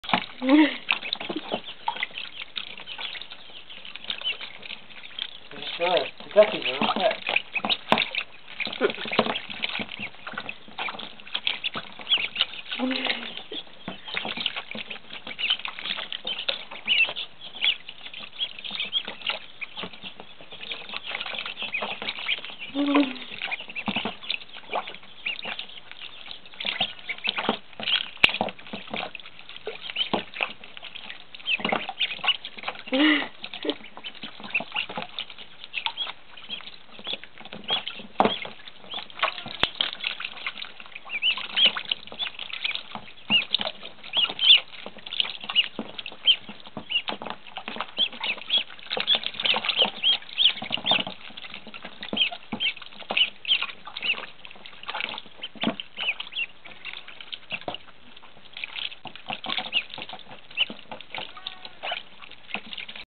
Duckies First Swim
My week and a half duckies first time swimming.  Baby Rouen and Pekin I think, I got them as a Ducky Surplus lot from Cackle Hatchery.